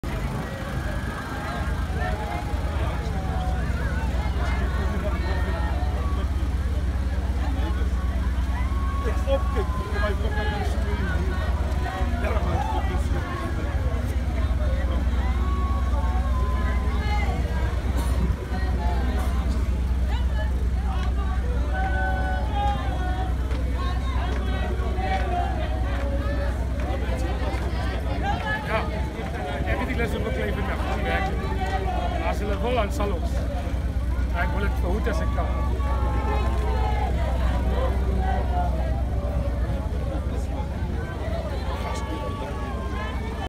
EFF in Kraaifontein 1
Economic Freedom Fighters protesting in Kraaifontein , Cape Town , South Africa